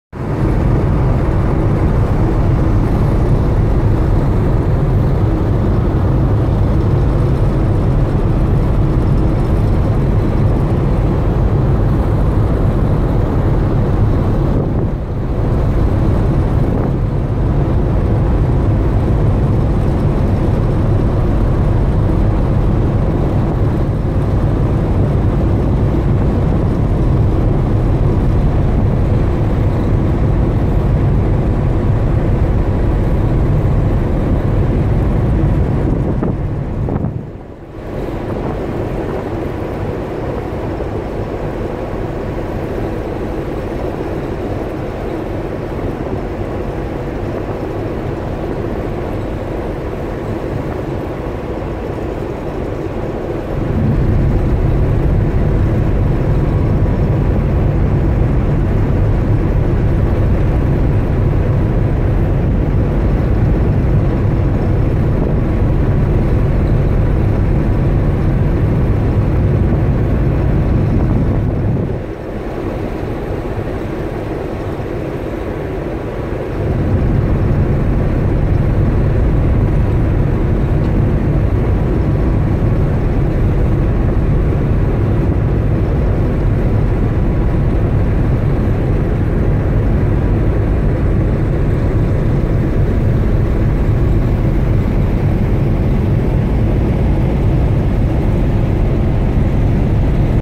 明石海峡大橋をくぐるジャンボフェリー
ジャンボフェリー（りつりん2）にて、高松港→神戸港
2018年12月31日17時50分頃に通過